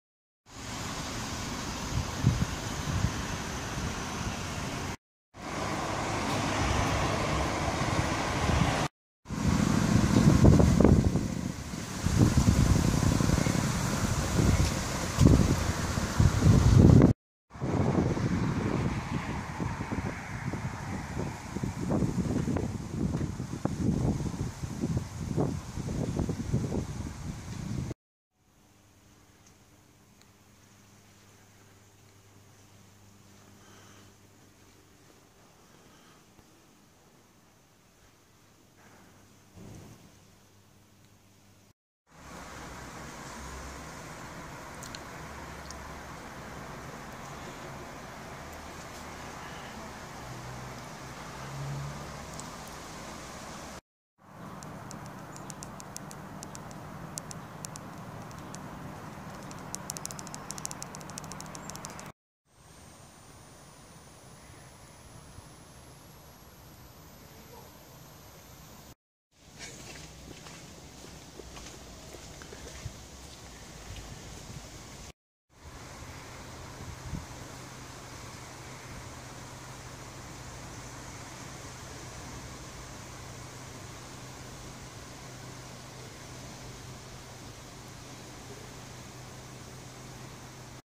Zur Rheha nach Königstein (Taunus)
Kurzer Besuch einer Rhea-KLinik in Königstein (Taunus)